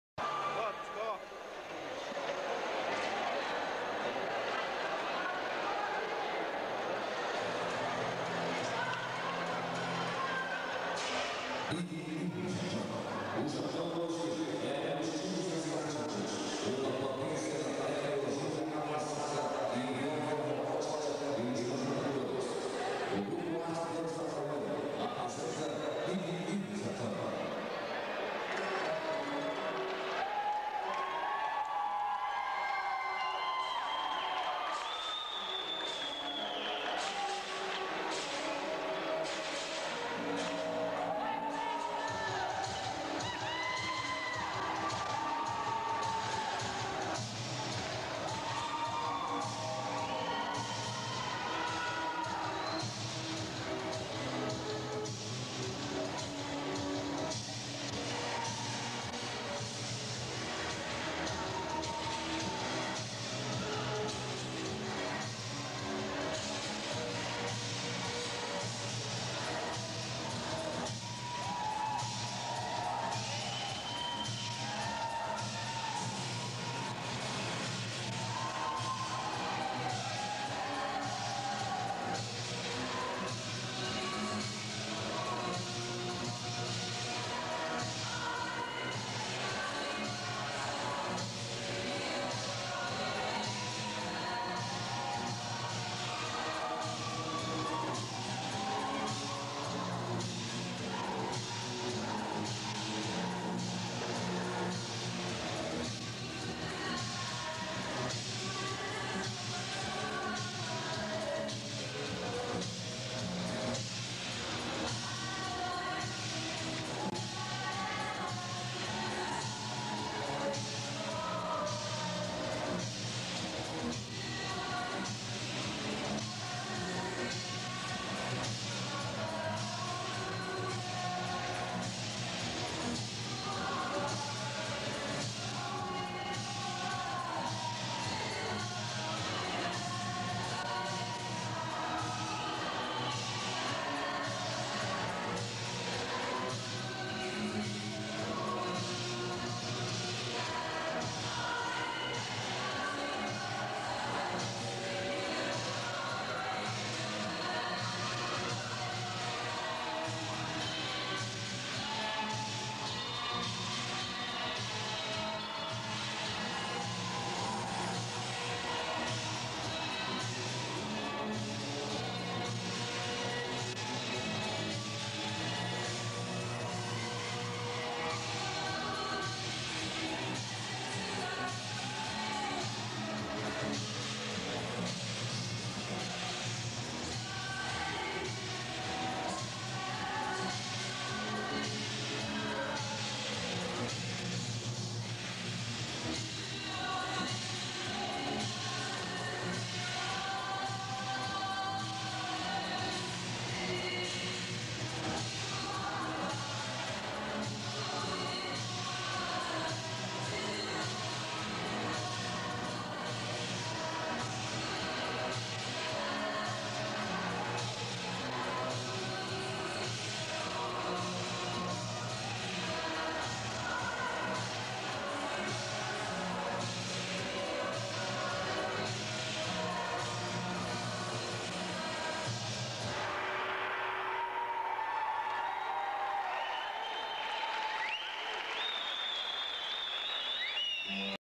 GRUPO ART DANÇA ABAIÃO 2ª DANÇA ACARÁ 2003
2ª DANÇA GRUPO ART DANÇA ABAIÃO REPRESENTANDO O MUNICÍPIO DE ABAETETUBA NO FESTIVAL INTERMUNICIPAL DE QUADRILHAS MODERNAS REALIZADO NO GINÁSIO DICO OLIVEIRA ACARÁ 2003.
AGRADECIMENTO MUITO ESPECIAL PARA FLÁVIO BRITO DE ABAETETUBA QUE CEDEU ESTE ARQUIVO PARA NOSSO ACERVO.